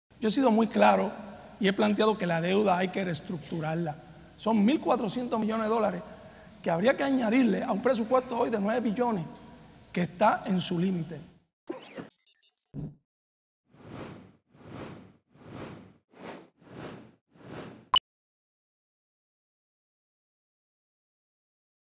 David Bernier sobre la reestructuración de la deuda
David Bernier: “Son $1,400 millones que habría que añadirle a un presupuesto hoy de $9,000 millones que está en un sumidero…”
Veredicto: Verdadero...